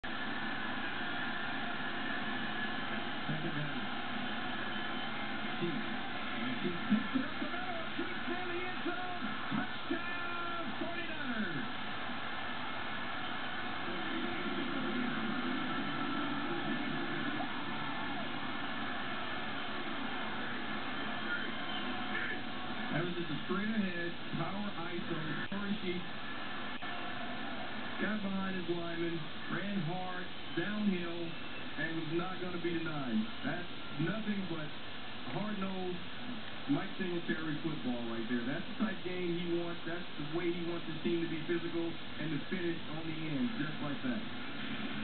49ers RB Kory Sheet Winning Touchdown - 49ers v. Cowboys 2009 Preseason
A determined Sheets explodes through a Cowboys linebacker and a couple of Safeties into the endzone for the winning touchdown... 8/29/09